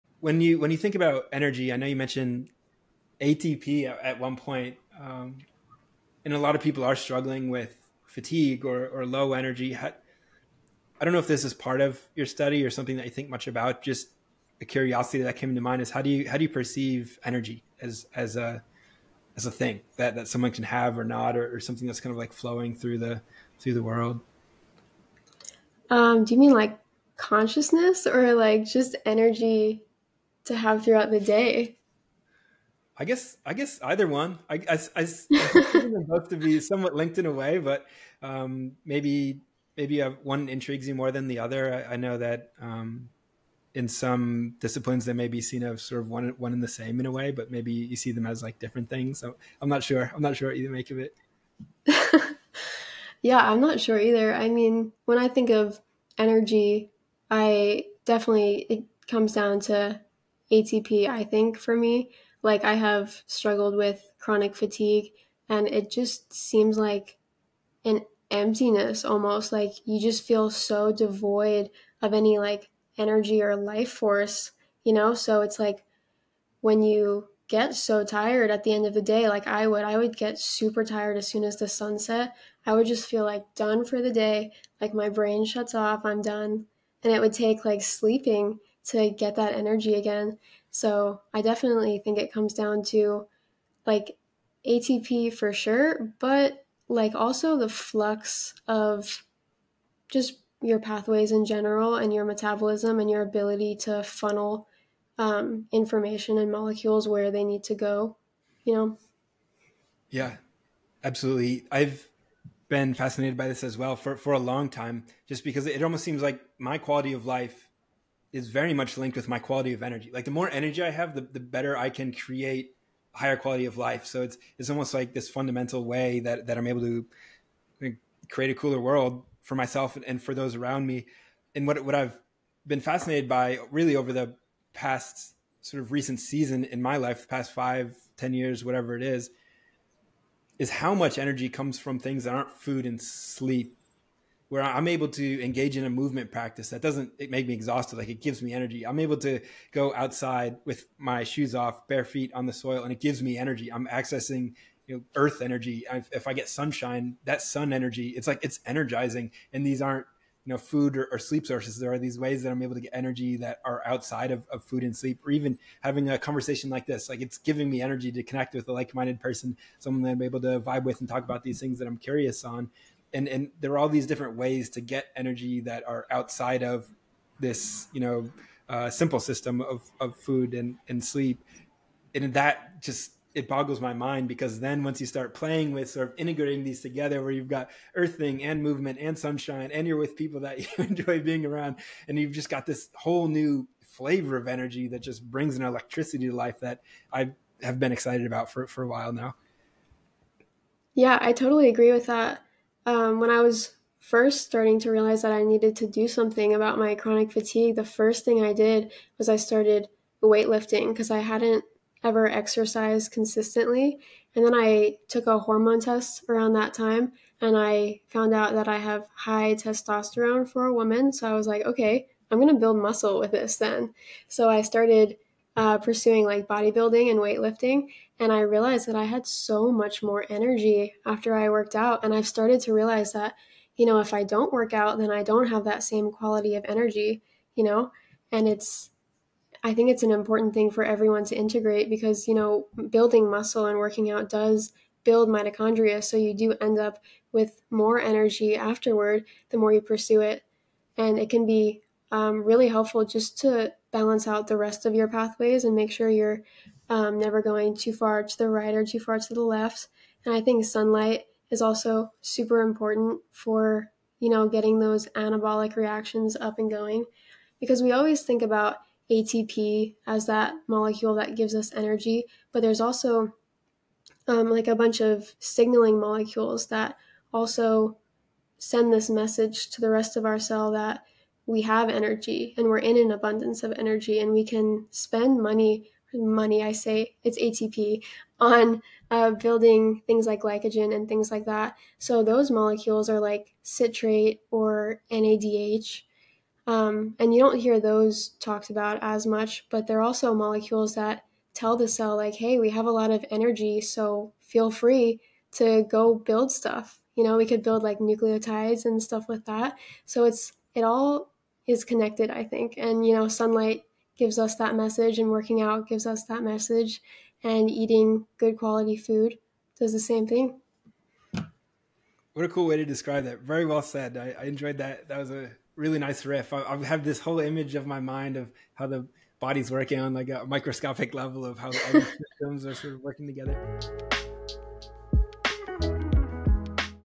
Overcoming Chronic Fatigue by Understanding ATP - Jessica Genetics | Peak Earth #37
Jessica @JessicaGenetics is a student, researcher, and scientist passionate about biology, genetics, health, and wellness. We discussed optimizing mitochondria, health habits, wellness principles, the power of learning, blue blocking glasses - and much more!